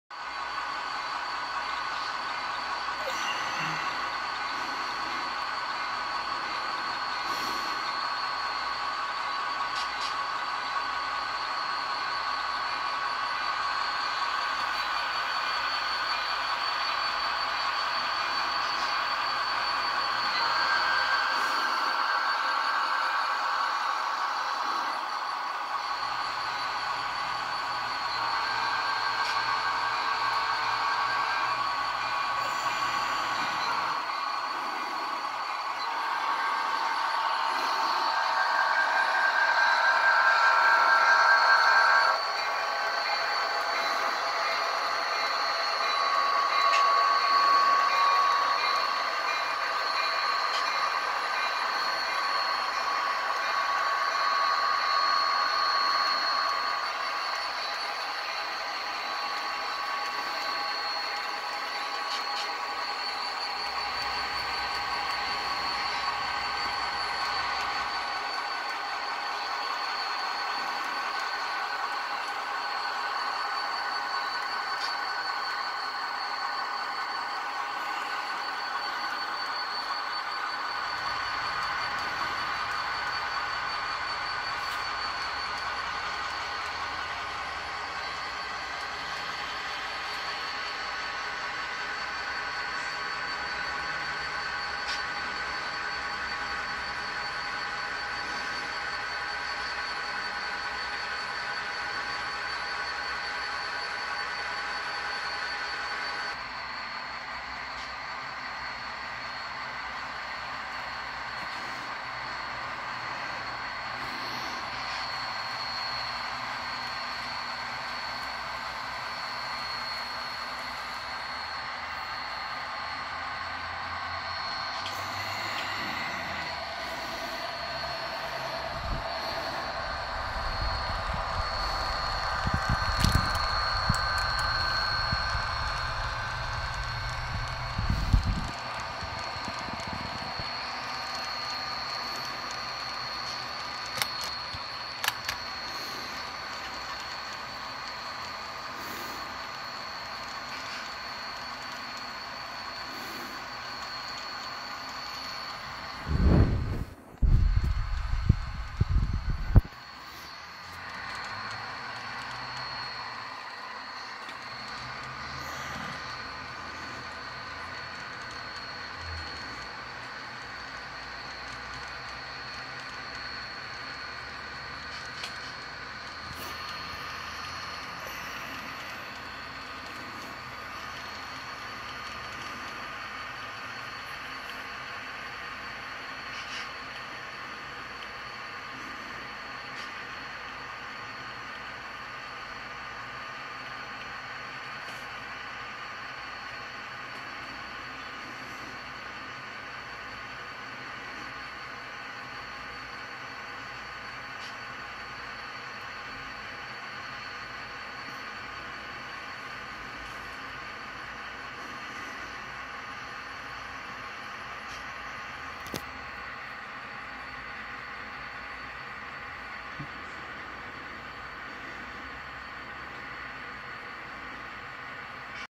Amtrak's Southwest Chief on the California Southern Model Railroad Club.
Rapido F40PHs and baggage car, Intermountain/Train Station Products Coach Dorm, and everything else is Kato.